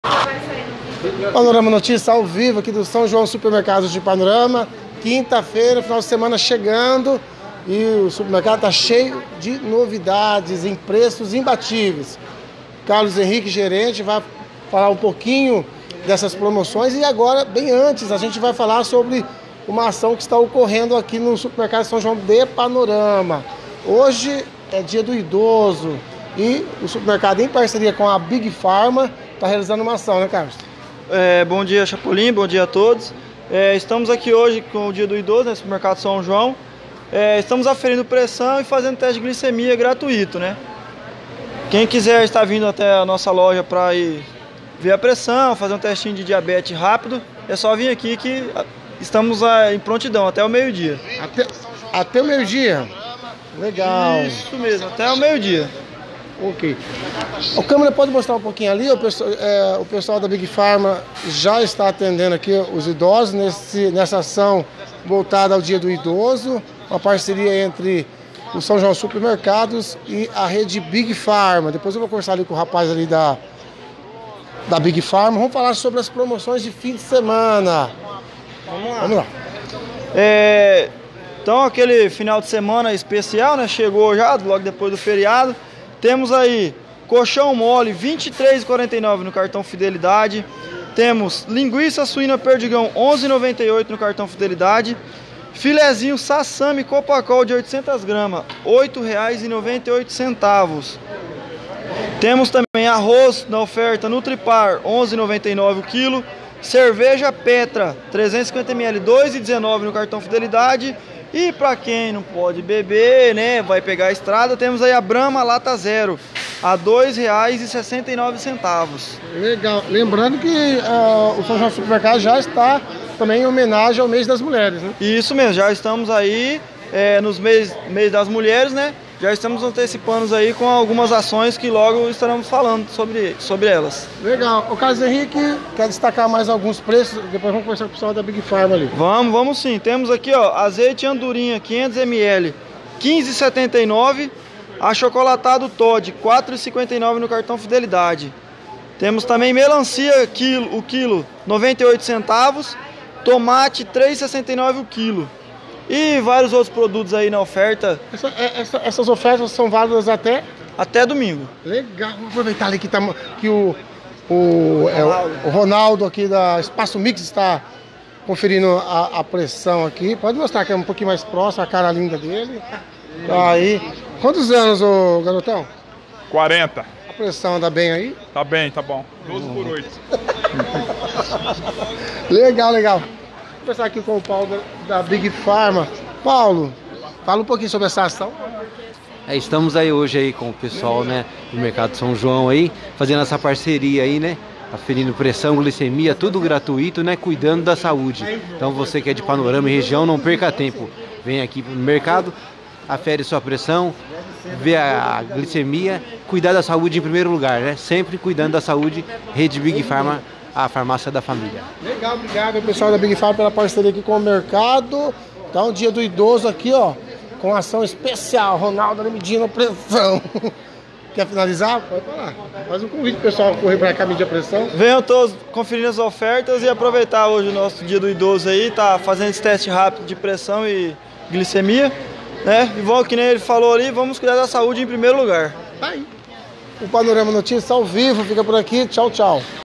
Dia do Idoso e promoções especiais de Fim de Semana no São João Supermercados de Panorama👏
Gravado manhã 27/02